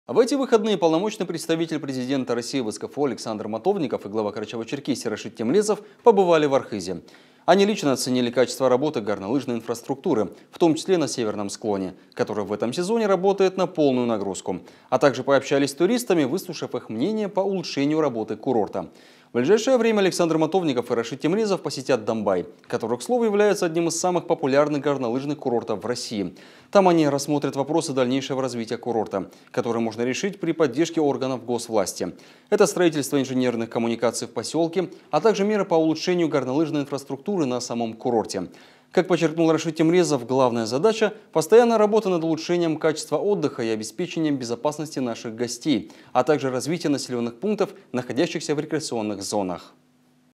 Александр Матовников и Рашид Темрезов обсудили развитие туристической инфраструктуры в КЧР
В эти выходные Полномочный представитель Президента России в СКФО Александр Матовников и Глава Карачаево-Черкесии Рашид Темрезов побывали в Архызе. Они лично оценили качество работы горнолыжной инфраструктуры, в том числе на Северном склоне, который в этом сезоне работает на полную нагрузку. А также пообщались с туристами, выслушав их мнение по улучшению работы курорта. В ближайшее время Александр Матовников и Рашид Темрезов посетят Домбай, который, к слову, является одним из самых популярных горнолыжных курортов в России. Там они рассмотрят вопросы дальнейшего развития курорта, которые можно решить при поддержке органов государственной власти. Это строительство инженерных коммуникаций в поселке, а также меры по улучшению горнолыжной инфраструктуры на самом курорте. Как подчеркнул Рашид Темрезов, главная задача — постоянная работа над улучшением качества отдыха и обеспечением безопасности наших гостей, а также развитие населенных пунктов, находящихся в рекреационных зонах».